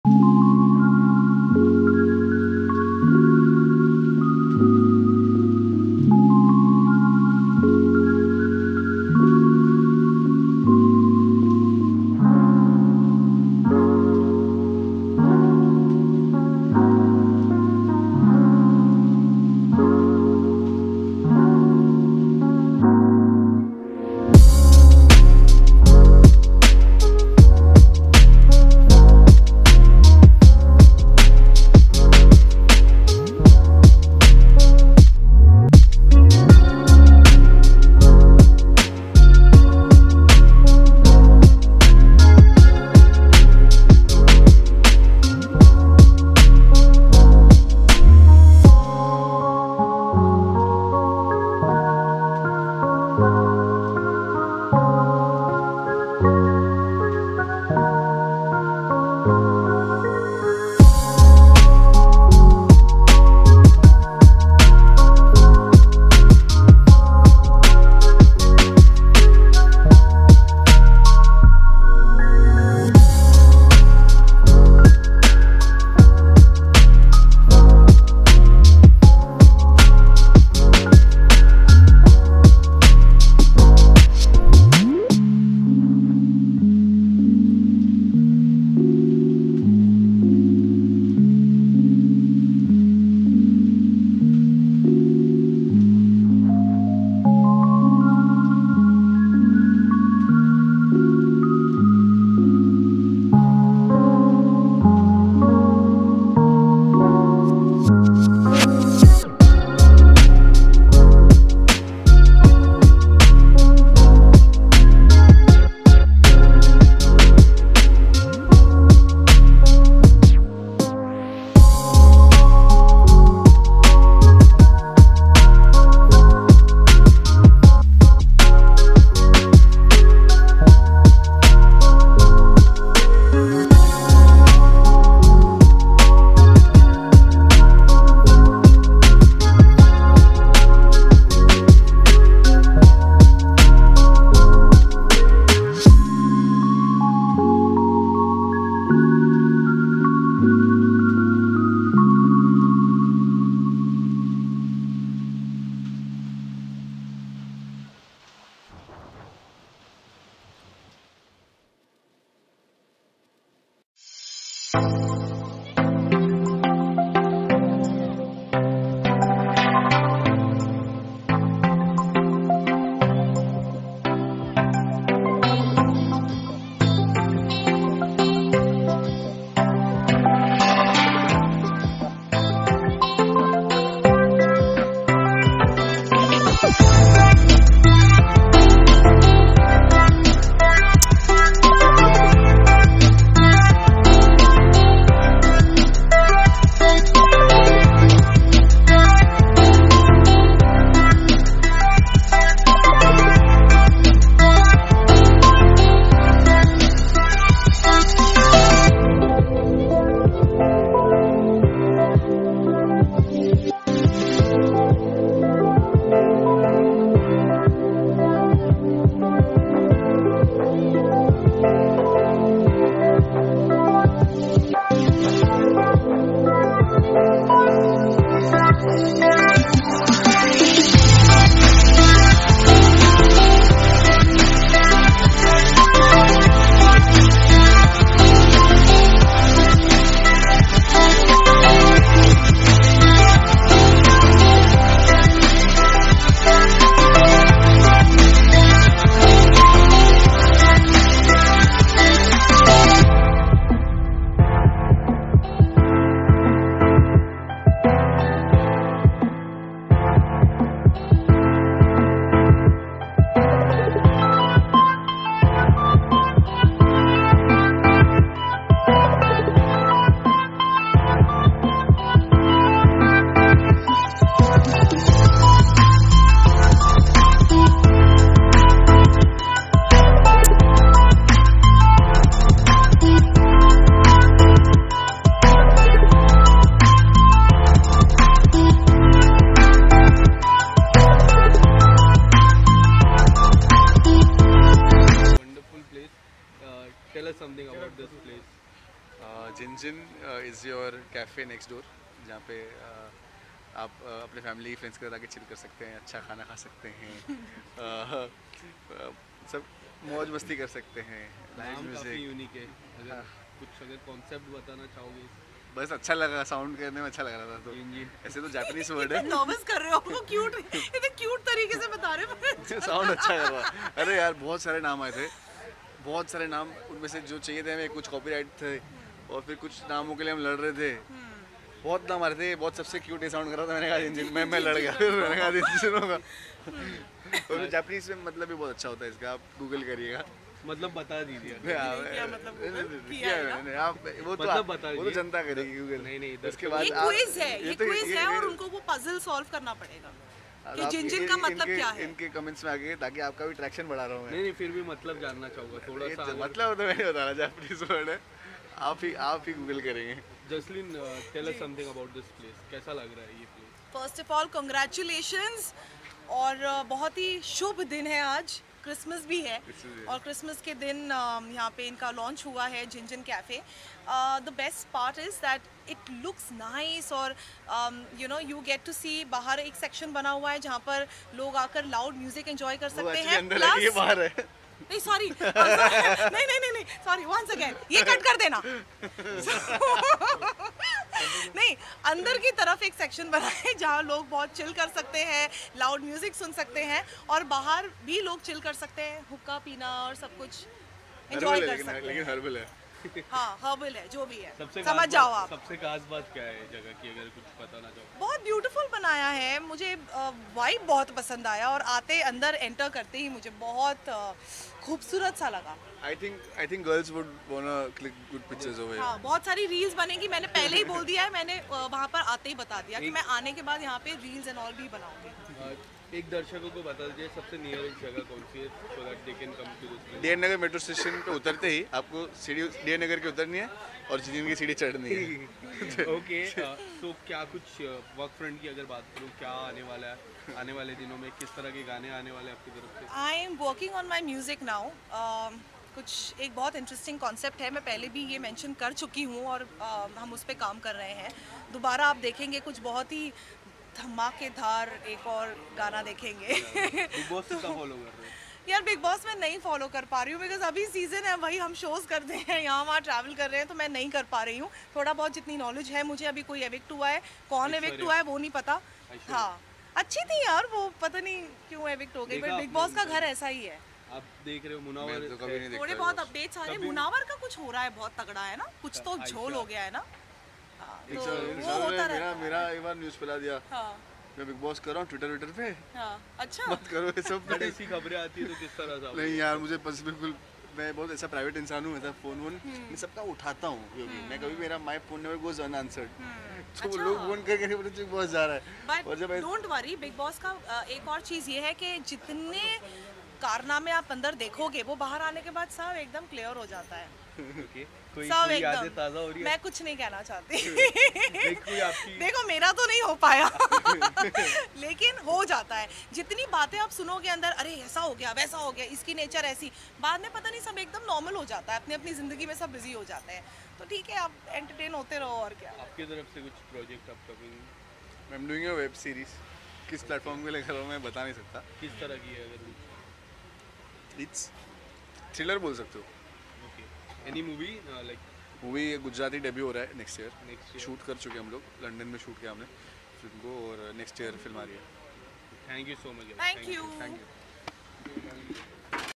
Jasleen matharu, Bhavin Bhanushali, Asif Rozani spotted at Gingin Cafe
welcome to em bollywood , your one stop  Destination For Everything and bollywood celebraty spotted, movie event, and  Celebrities & Indian Television Stars